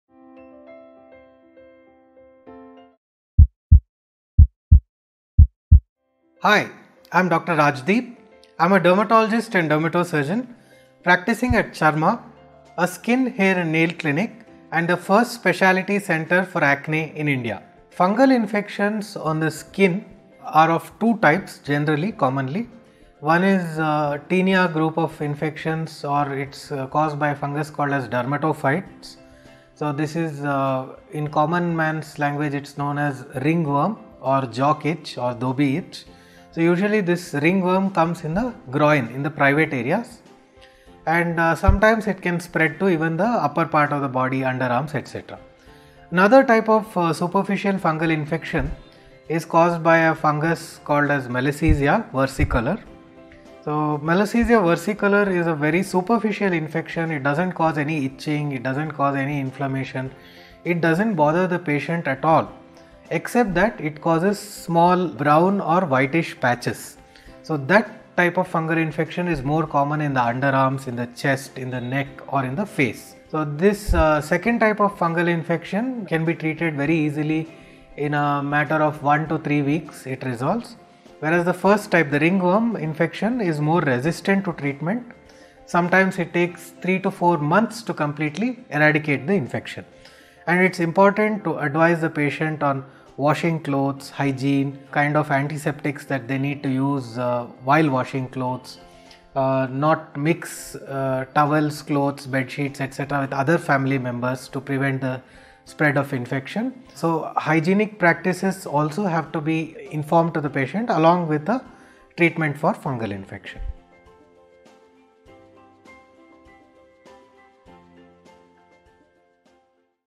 Underarm Fungal Infection: Causes, Treatment & Prevention - Dr. Rajdeep Mysore | Doctors' Circle
Dr. Rajdeep Mysore | Appointment booking number: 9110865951
Consultant Dermatologist & Dermatosurgeon | Charma Chirag Hospital, Bangalore
Fungal Infection on the  skin are  of  two  types. One is  tenia  group of  infection  or  caused  by a    fungus called  as  dermatophytes. So this is   in common mans  language   known  as  Ringworm  or  Jock  itch or Dhobie  Itch. So usually  this Ringworm  comes  in the  groin  or  in the private  areas  and sometimes  it  can spread even to the  upper part of the  body or  the underarms. Another superficial infection is caused by Malsezia versicolor.  So Malassezia versicolor is a very superficial infection and it  doesn’t  cause  any itching inflammation and doesn’t  bother the  patient  at all, except that it  can  cause  brown or  whitish patches.   So that type of Fungal Infection is more common in the underarms., the  chest, neck or  in the face. This second type  of Fungal Infection  can be  treated  very  easily   in about   1 to 3  weeks  whereas the  first  type that is the ringworm is  more  resistant  to  treatment sometimes it  takes  about   3  to  4 months   to completely  eradicate  the  infection. It  is  important to  advise  patient  on  washing  clothes,  hygiene  kind of  antiseptics  they need  while  washing clothes. Not to mix towels clothes  with other  family members to prevent  the spread of  infection. So hygienic  practices  also    has to  be informed to the  patient  also  with the  Fungal Infection.